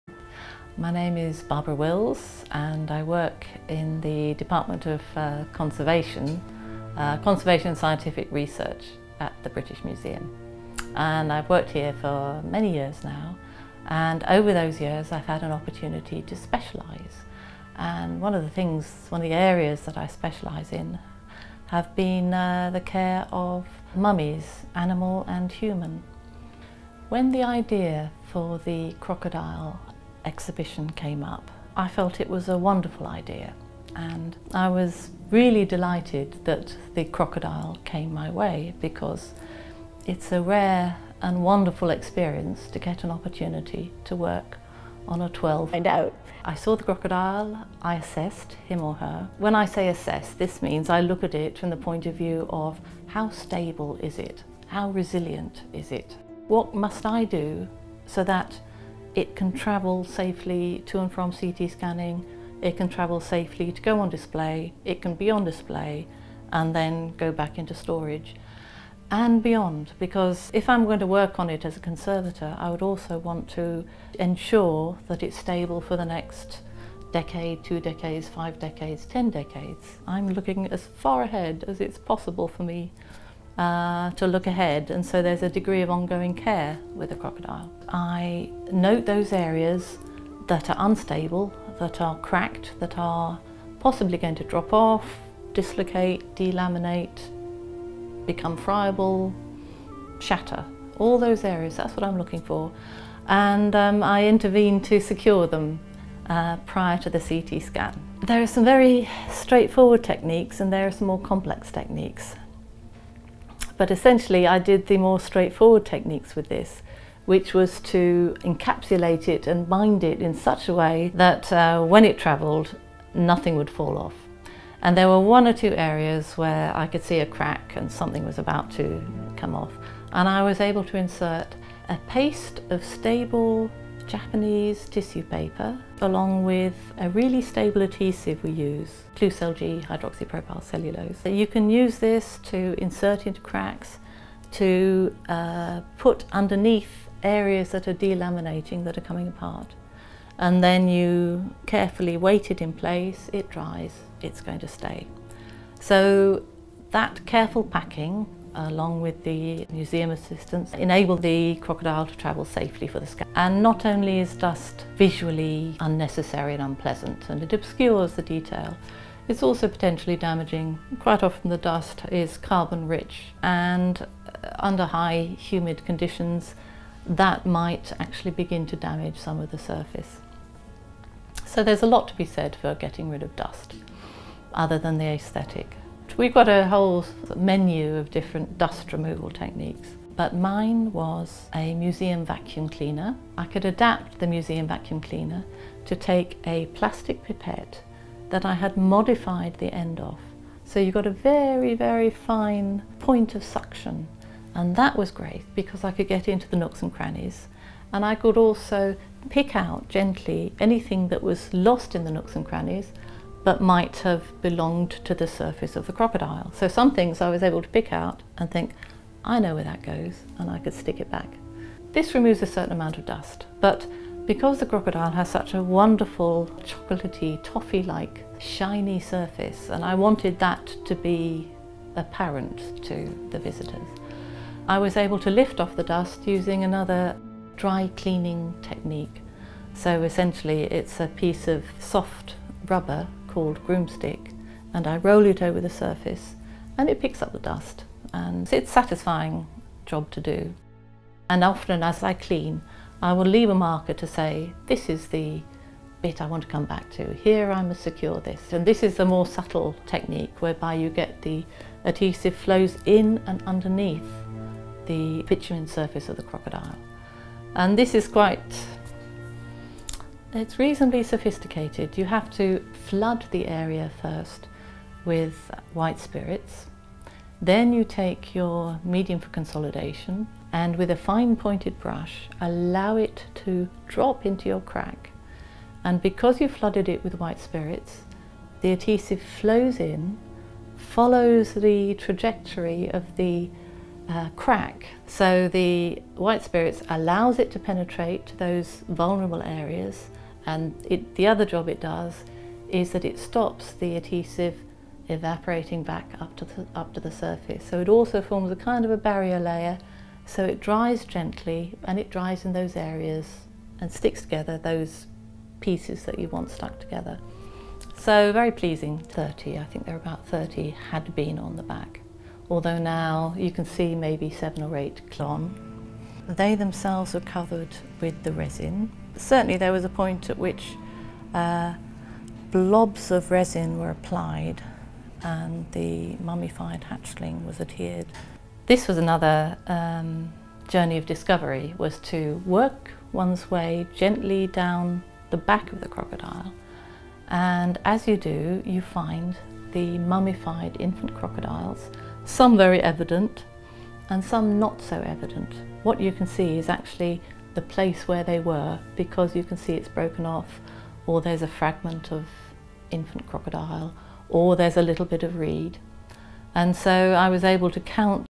Crocodile Conservationist Barbara Wills - British Museum
For ASMR purposes I have edited this video to just show all of barbara's scenes, as I found her voice very soothing.